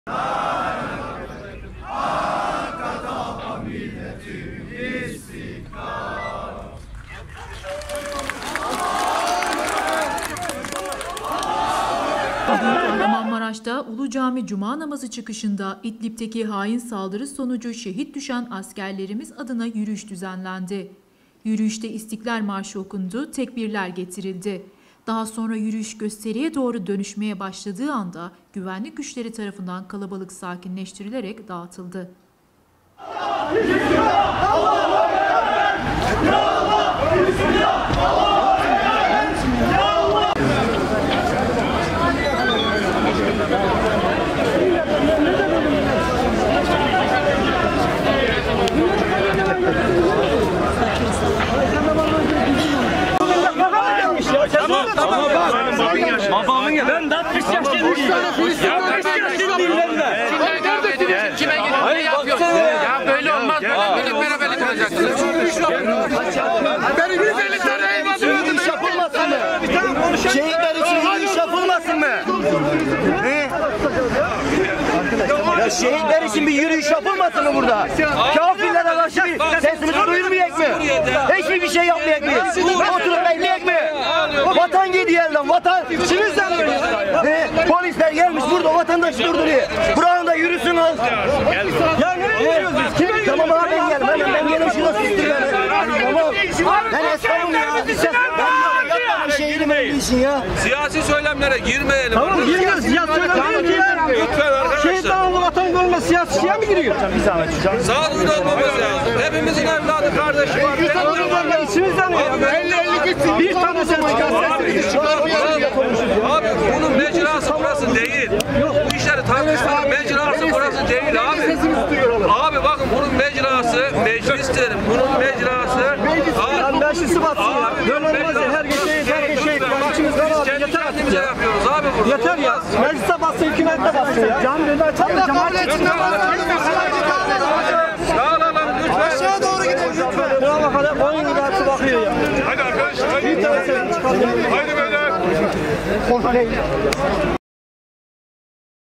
KAHRAMANMARAŞ'TA ULUCAMİ ÇIKIŞI İDLİB ŞEHİTLERİ İÇİN YÜRÜYÜŞ YAPILDI!
Bugün Kahramanmaraş'ta Ulucami Cuma namazı çıkışında İdlib'deki hain saldırı sonucu şehit düşen askerlerimiz adına yürüyüş düzenlendi.